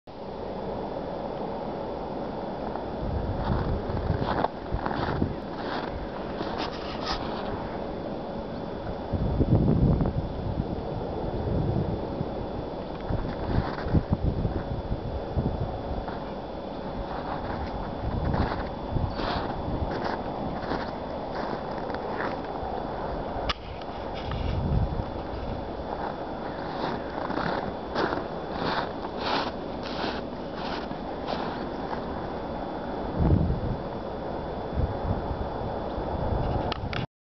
12.12.2012 в Строгино
Берег Москва-реки утром(9.35)